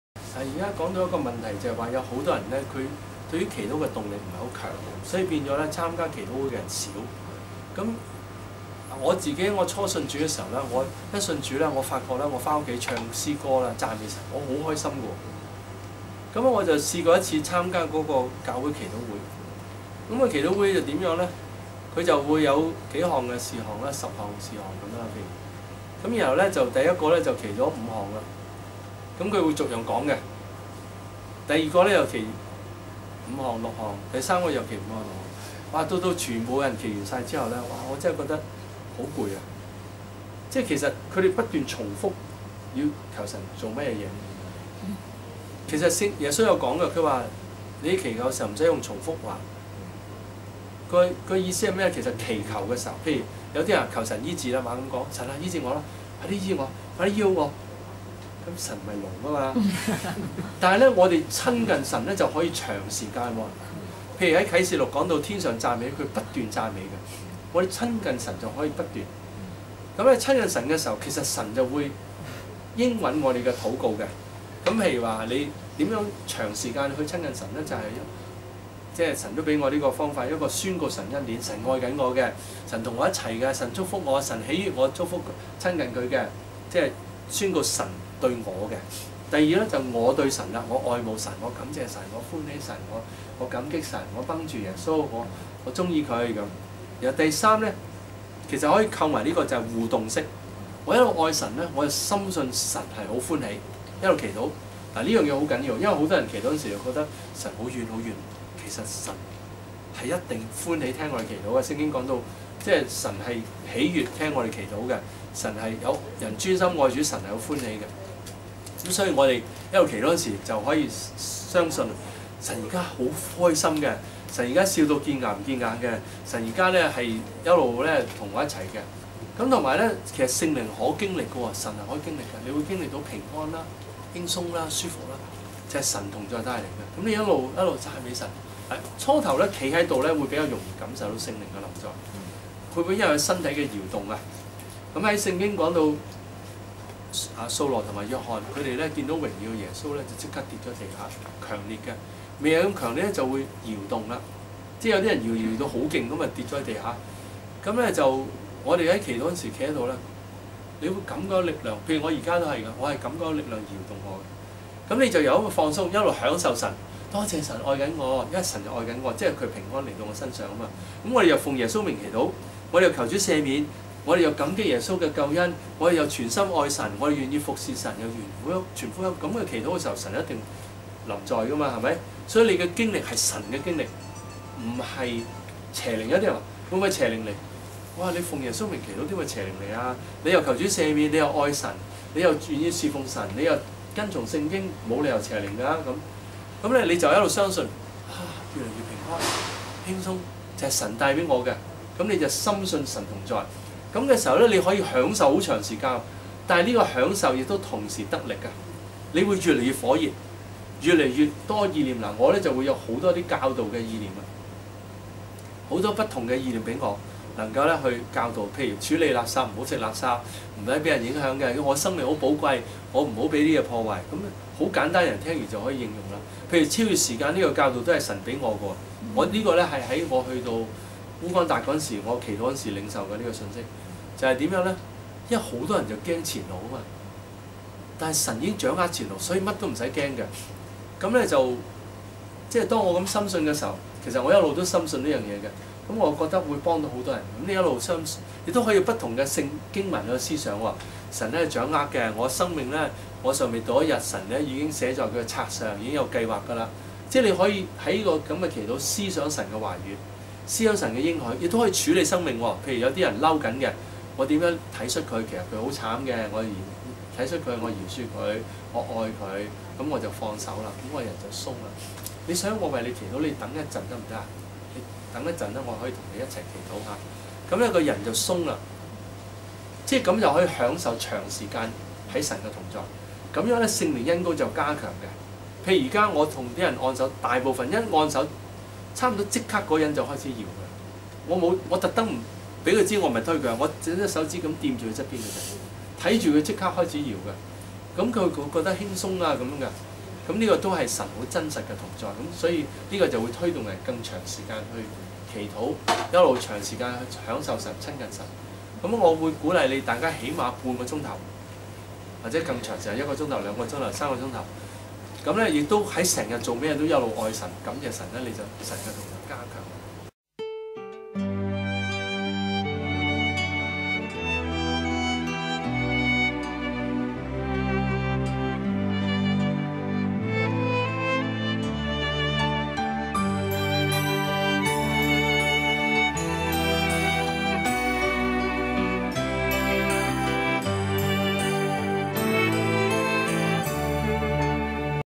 如何提升祈禱的動力 - 葉牧師
如何提升祈禱的動力
1．星期日：3:15崇拜；2:30服事禱告；崇拜後有小組及侍奉訓練課程。
2．星期一：4時服事禱告，7時侍奉訓練課程。